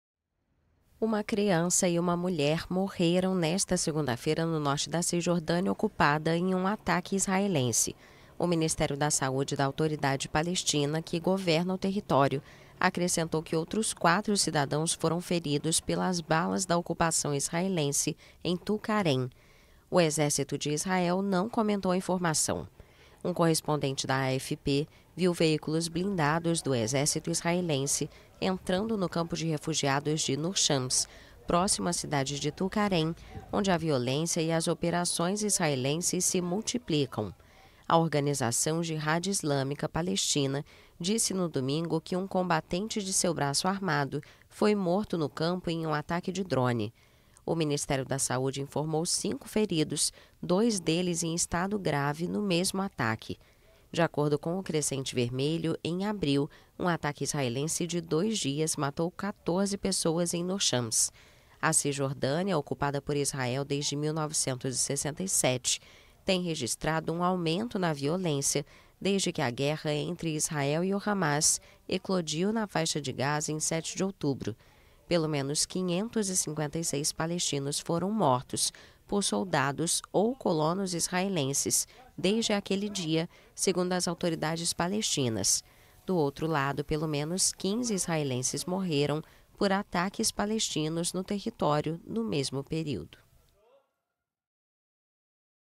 Criança e mulher morrem na Cisjordânia em ataque israelense | AFP
A Autoridade Palestina anunciou que uma mulher e uma criança morreram na Cisjordânia em um ataque israelense. O território vive um aumento da violência desde o início da guerra entre Israel e o Hamas em 7 de outubro.